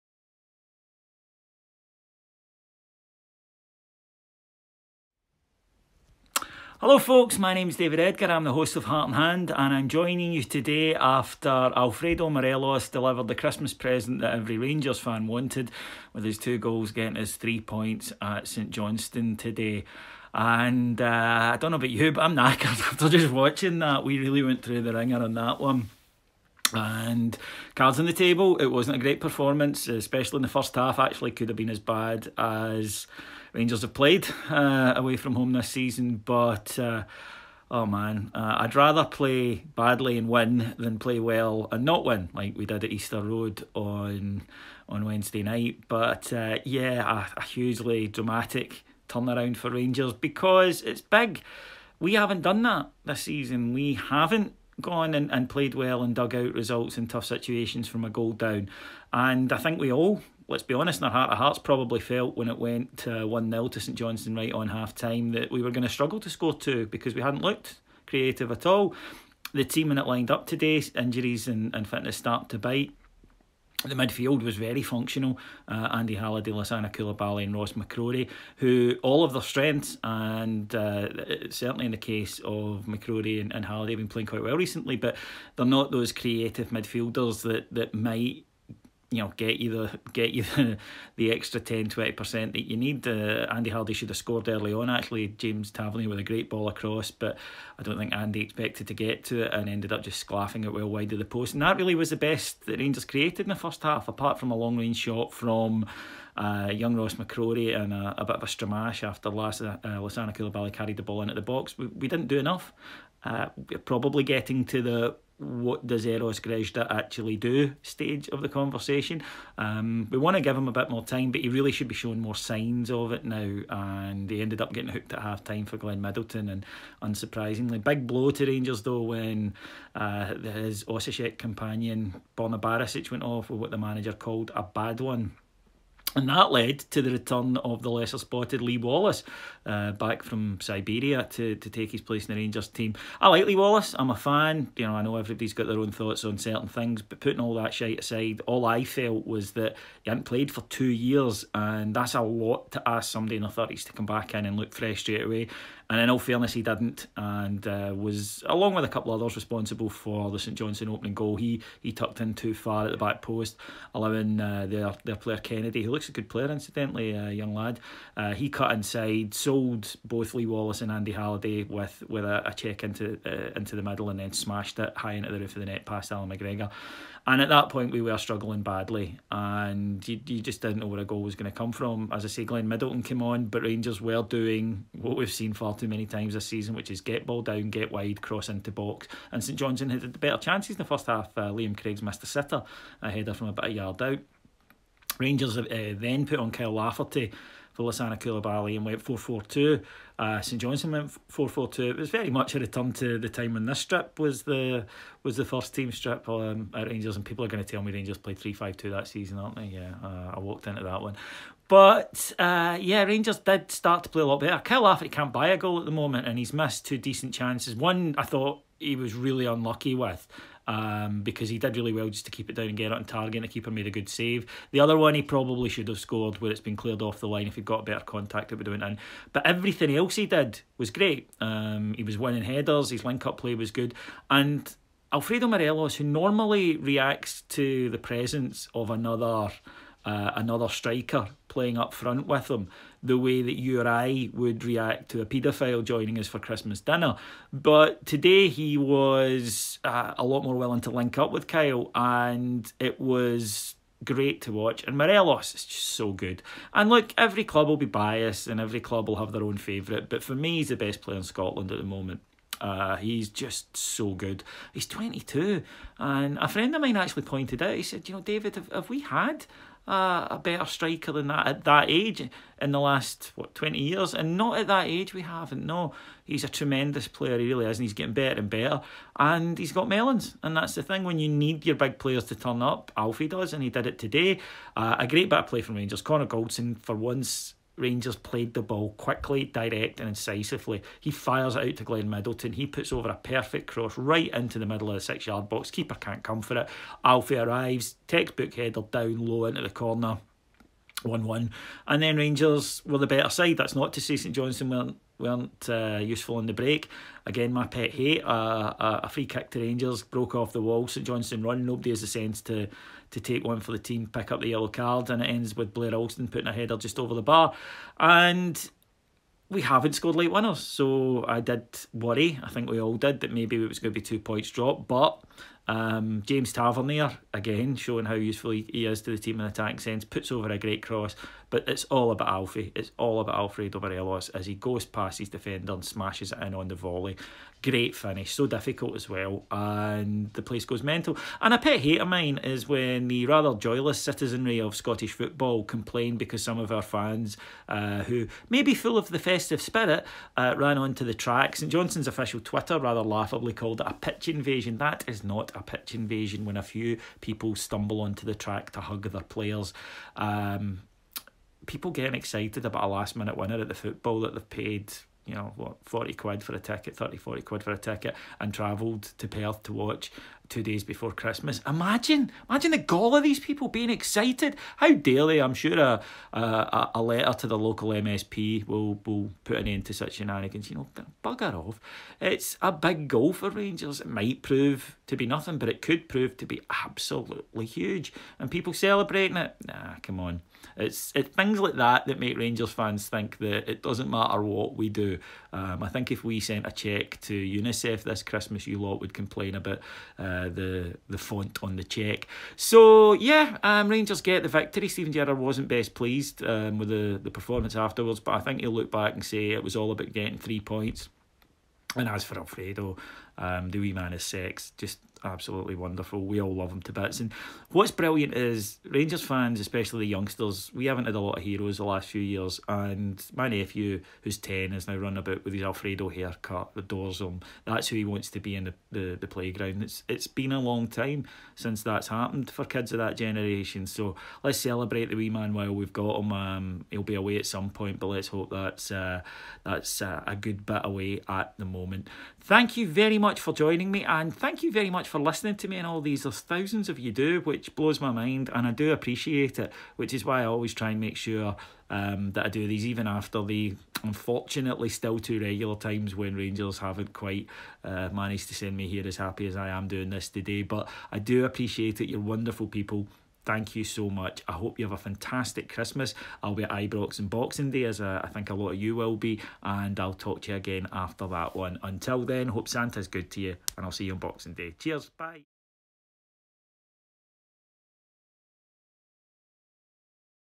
St. Johnstone v Rangers Postmatch Reaction 23 Dec 2018
Santa Alfie delivers three points! David pays tribute to El Buffalo.